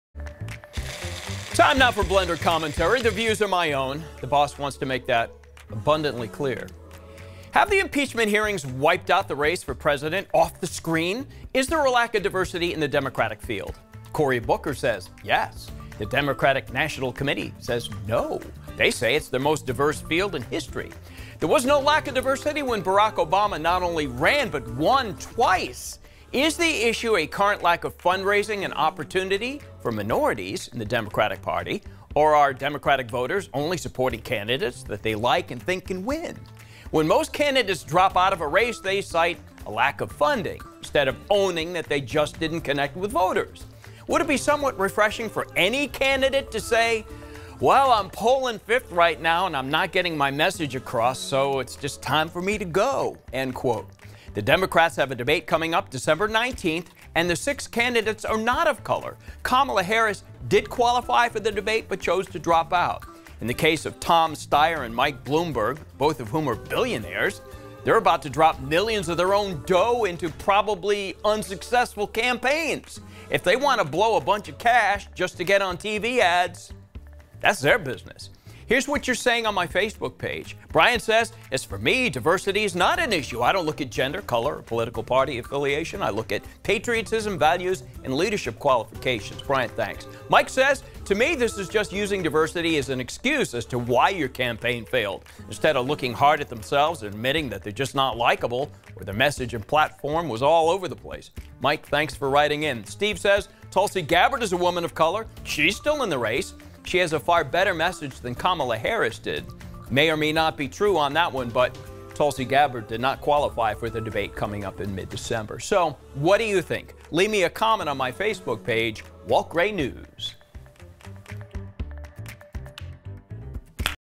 Walt's Blender: Is there a lack of diversity in the Democratic field for President?
Is there a lack of diversity in the Democratic field?
Cory Booker says yes. The Democratic National Committee disagrees. They say it's their most diverse field.